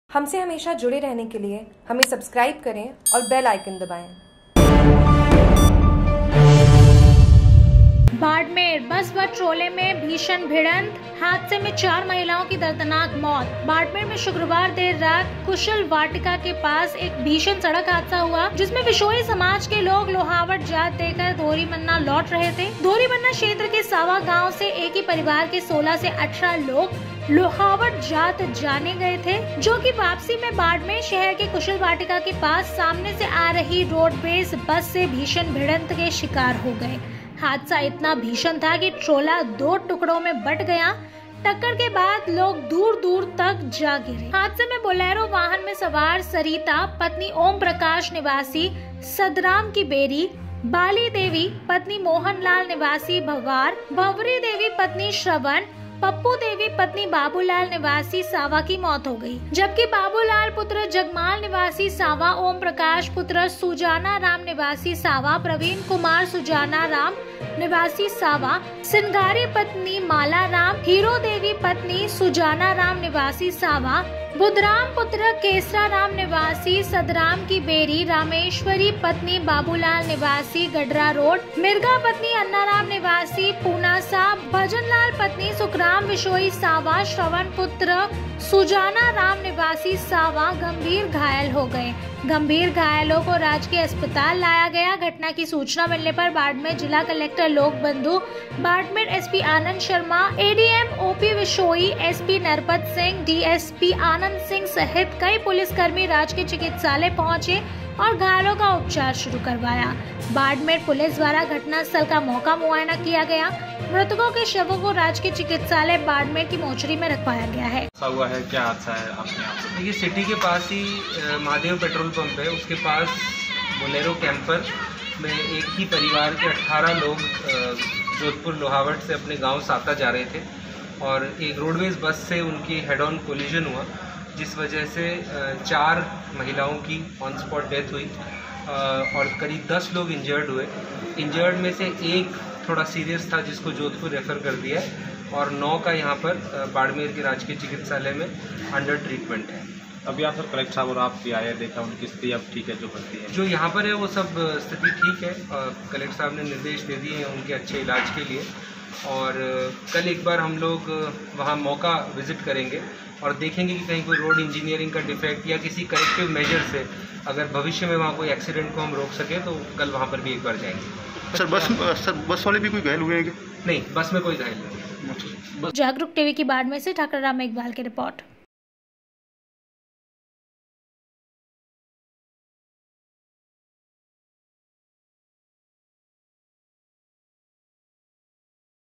बाड़मेर : बस व ट्रोले में भीषण भिड़ंत, हादसे में चार महिलाओं की दर्दनाक मौत
बाड़मेर में शुक्रवार देर रात कुशल वाटिका के पास एक भीषण सड़क हादसा हुआ। जिसमें विश्नोई समाज के लोग लोहावट जात देकर धौरीमन्ना लौट रहे थे।

धोरीमन्ना क्षेत्र के सावा गांव से एक ही परिवार के 16-18 लोग लोहावट जात देने गए थे। जो कि वापसी में बाड़मेर शहर के कुशल वाटिका के पास सामने से आ रही रोडवेज बस से भीषण भिड़ंत के शिकार हो गए।

हादसा इतना भीषण था कि ट्रोला दो टुकड़ों में बंट गया। टक्कर के बाद लोग दूर-दूर तक जा गिरे। हादसे में बोलेरो वाहन में सवार सरिता (20) पत्नी ओमप्रकाश निवासी सदराम की बेरी, बालीदेवी (25) पत्नी मोहनलाल निवासी भंवार, भंवरीदेवी (22) पत्नी श्रवण, पप्पूदेवी (40) पत्नी बाबूलाल निवासी सांवा की मौत हो गई।

जबकि बाबूलाल पुत्र जगमाल निवासी सांवा, ओमप्रकाश पुत्र सुजानाराम निवासी सांवा, प्रवीण पुत्र सुजानाराम निवासी सांवा, सिणगारी पत्नी मालाराम, हीरोदेवी पत्नी सुजानाराम निवासी सांवा, बुद्धराम पुत्र केसराराम निवासी सदराम की बेरी, रामेश्वरी पत्नी बाबूलाल निवासी गडरारोड़, मिरगा पत्नी अन्नाराम निवासी पूनासा, भजनलाल पत्नी सुखराम निवासी सांवा, श्रवण पुत्र सुजानाराम निवासी सांवा गंभीर घायल हो गए